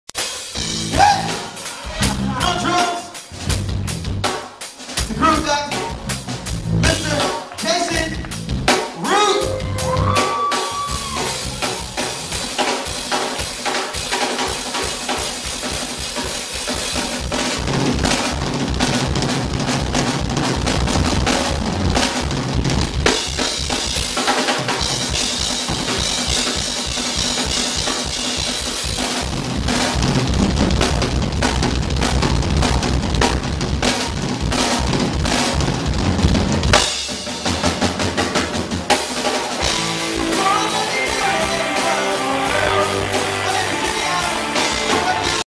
Jason Root - Drum Solo - 9/2010
Jason Root - performing live on stage with Jimmie Allen (American Idol) in 2010